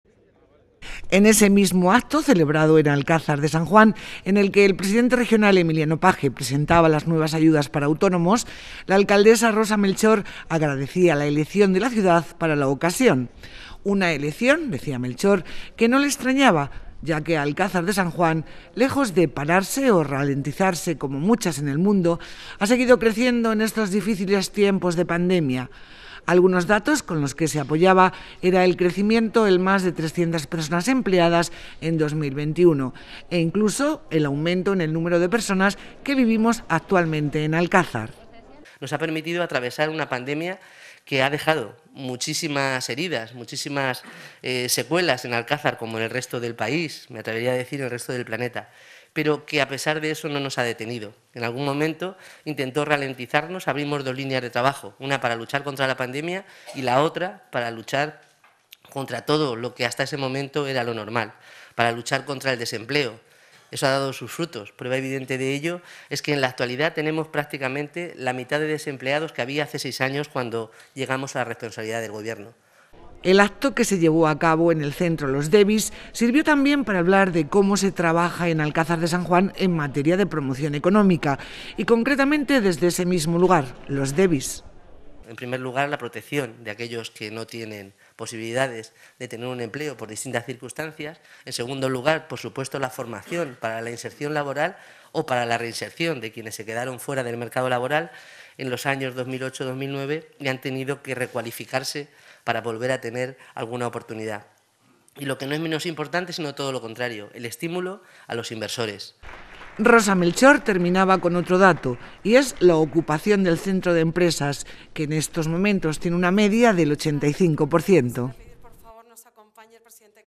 Rosa Melchor en el acto de ayudas para Autónomos
El acto que se llevó a cabo en el Centro Los Devis,   sirvió también para hablar de cómo  se trabaja  en Alcázar de San Juan  en materia Promoción económica  y concretamente desde ese mismo lugar, Los Devis